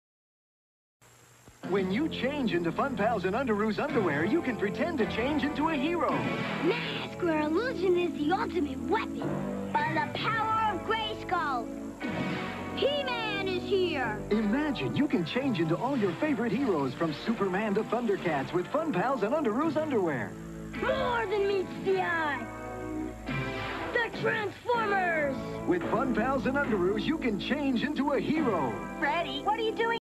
Optimus Prime...en ropa interior...
Cuando piensas en comprar ropa interior, piensas en tus héroes de infancia,piensas en Optimus Prime...
When you think of buying underwear, you think of your childhood heroes, you think of Optimus Prime...